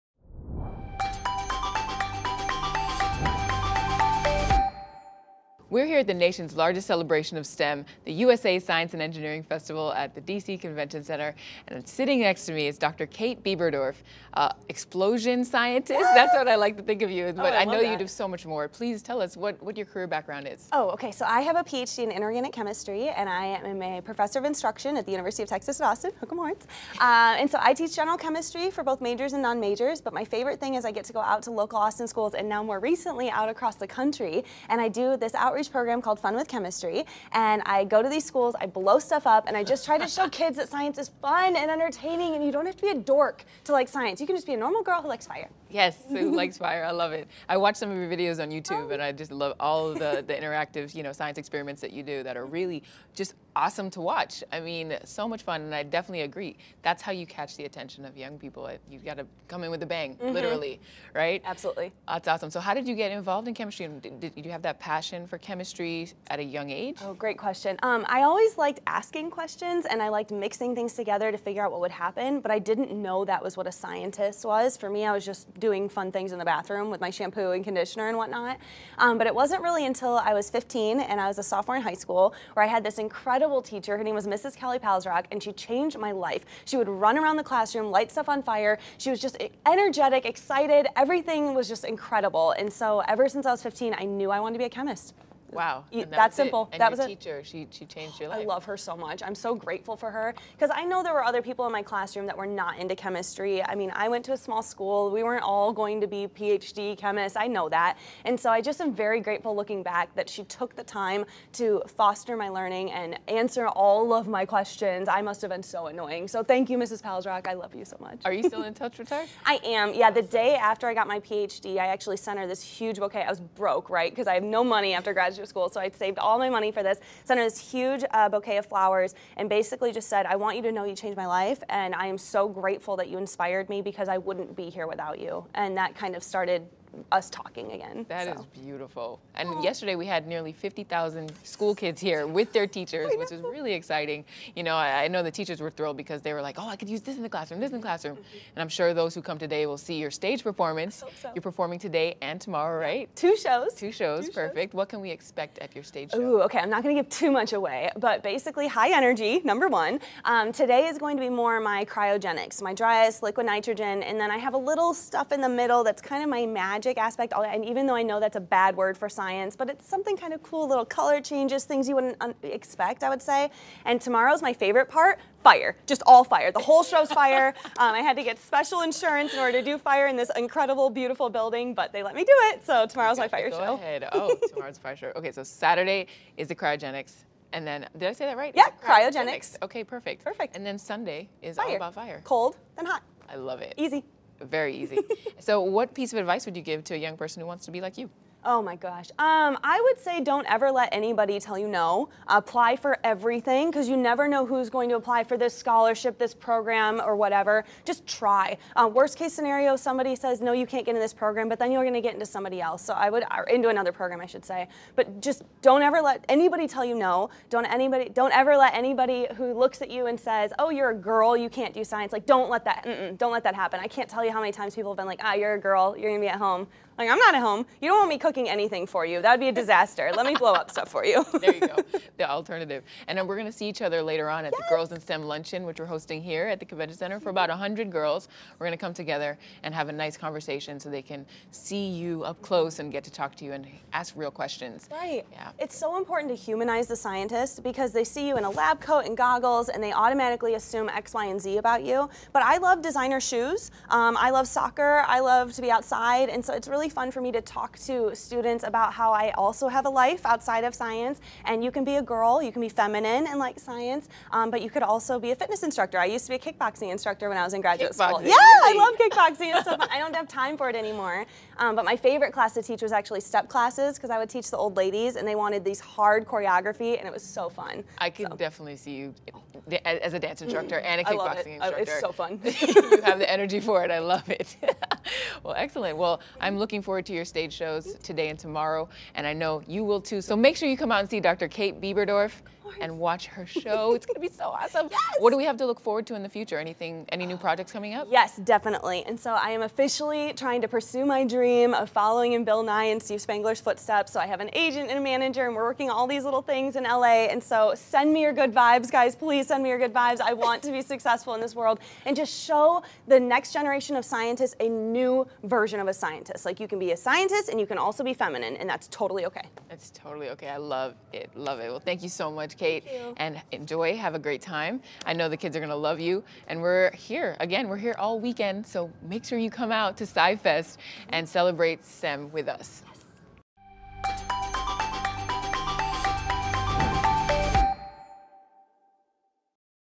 From the Festival Floor with Chemist Kate Biberdorf!
From the Festival Floor, Danni Washington interviews Dr. Kate Biberdorf from Fun with Chemistry about her Explosive Science Show here at SciFest!